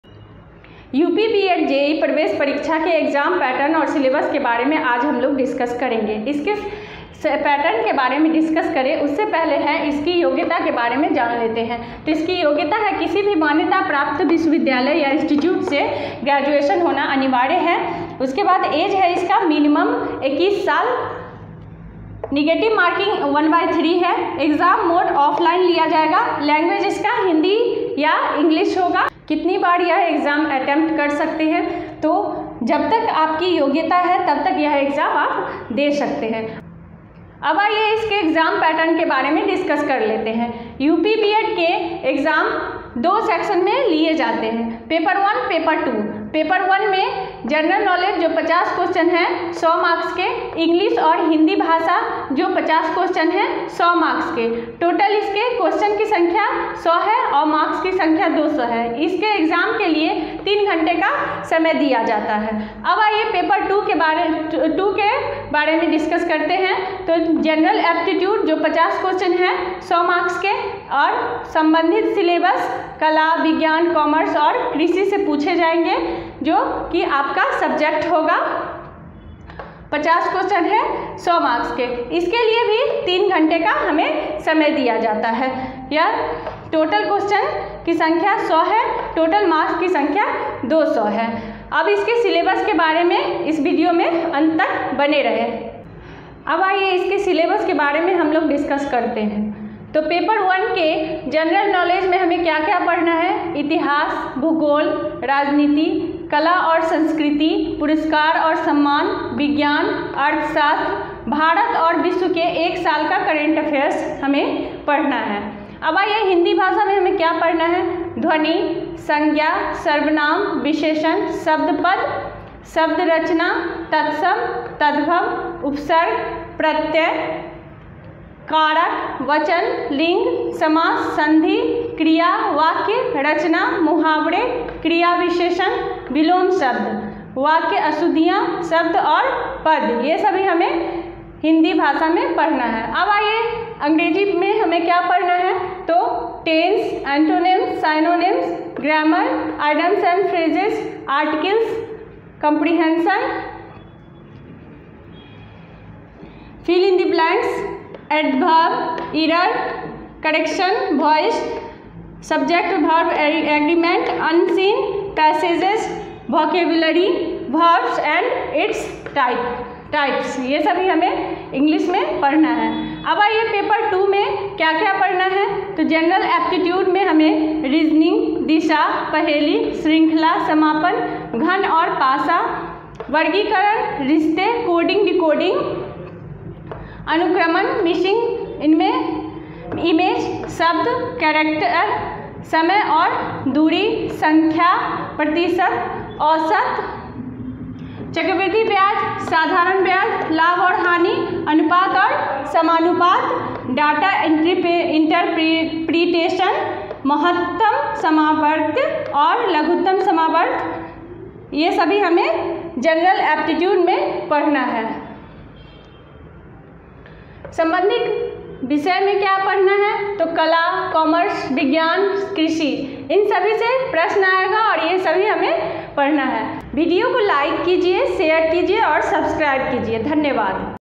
उत्तर प्रदेश Up B.Ed एंट्रेंस Exam Syllabus, Uttar Pradesh BEd Entrance Exam Pattern, Eligibility
At Mysyllabus you can find latest Syllabus, Exam Pattern, Eligibility Criteria of UP B.Ed Exam, Uttar Pradesh Pre BEd Entrance Exam.